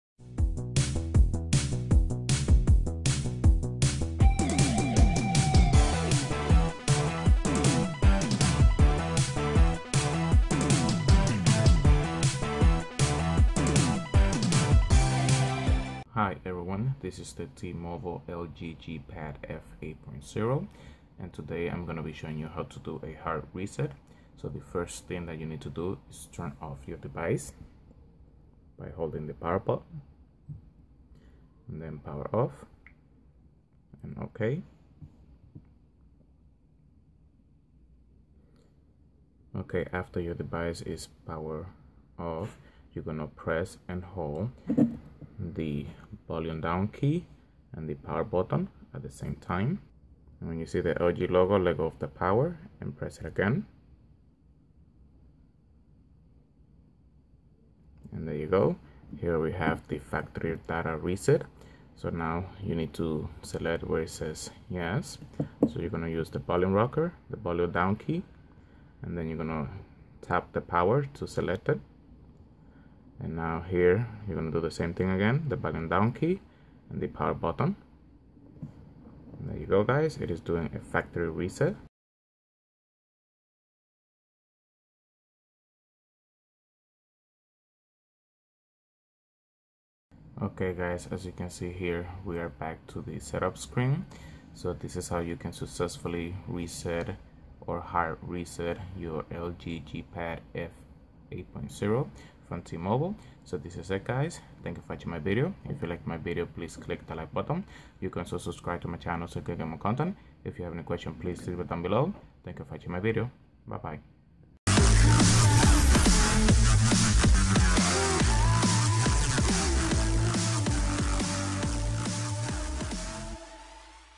T-Mobile LG G Pad F 8.0 Hard Reset
Master reset with hardware keys
A master reset restores the original factory settings and may delete your personal data on the internal storage, such as downloads, ringtones, images, apps, contacts, and Visual Voicemail. It does not delete data stored on the SIM card or SD card.

If the device menus are frozen or unresponsive, you can master reset using hardware keys. To perform a master reset, follow these steps:
1) Back up all data on the internal memory.
2) Turn the power off.
3) Press and hold the Power + Volume Down key on the right side of the device until the LG logo is displayed.
4) Briefly release only the Power key when the LG logo is displayed.
5) Quickly press and hold the Power key again.
6) Release all keys when the Factory reset screen is displayed.
7) Press the Volume keys to highlight Yes, then the Power key to select factory reset.
8) Press the Volume keys to highlight Yes, then the Power key to start the reset.
9) The device will automatically restart.

Master reset from settings menu

To perform a master reset, follow these steps:
1) Back up all data on the internal memory.
2) From any home screen, tap Apps.
3) Tap Settings.
4) Under PERSONAL, tap Backup & reset.
5) Select or clear the following check boxes:
*Back up my data
*Automatic restore
6) Tap Backup account and follow the steps to create Google backup account, if necessary.
7) Tap Factory data reset.
8) Review the warning message and select or clear Erase SD card.
9) Tap Reset tablet then Next.
10) Tap Delete all.
11) Tap OK.